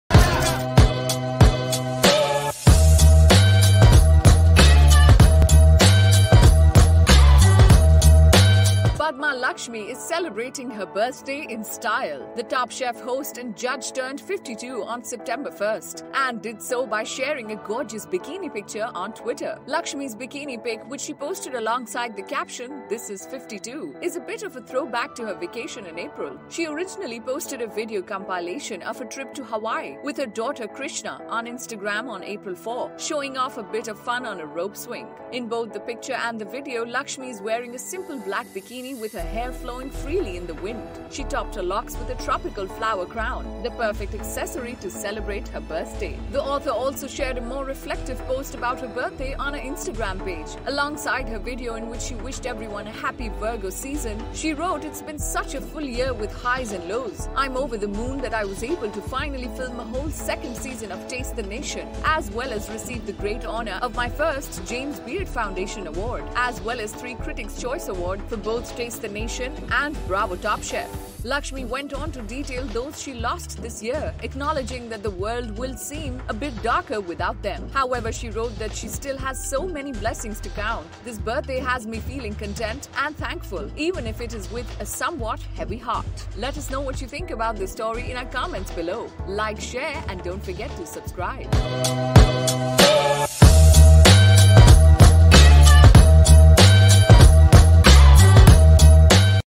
Padma Lakshmi Celebrates Her Birthday with a Beautiful Bikini Photo
Padma Lakshmi is celebrating her birthday in style.The Top Chef host and judge turned 52 on Sept. 1 and did so by sharing a gorgeous bikini picture